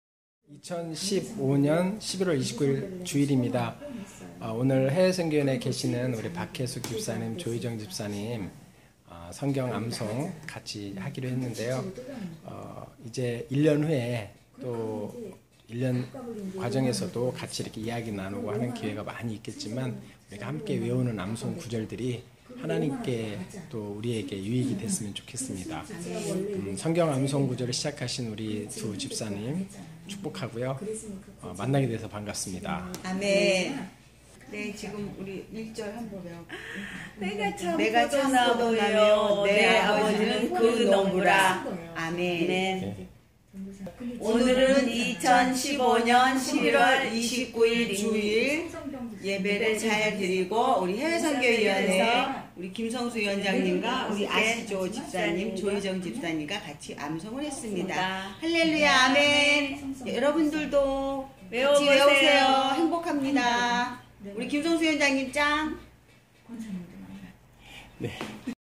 요한복음15장 암송과묵상 함께하며 주님을기쁘시게 권면합니다 김성수장로올림 인천순복음교회
요한복음15장 암송과묵상 함께하며 주님을기쁘시게 권면합니다 김성수장로올림 인천순복음교회 해외선교위원회 집사님들과 시작합니다

질문
1.성경을 함께암송하고 싶어요
2.요한복음15장 1절부터 10절까지 2016년 한해동안 하시죠
3.동의하시면 또다른 한분에게 권면해주세요

인류구원과 인류행복을위하여
성경7효와 7행복으로  하모니하는 세상을 만들어 갑시다
with the Bible 7  hyo and the 7 happiness!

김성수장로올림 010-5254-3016
신한은행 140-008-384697 (인천순복음 해외선교)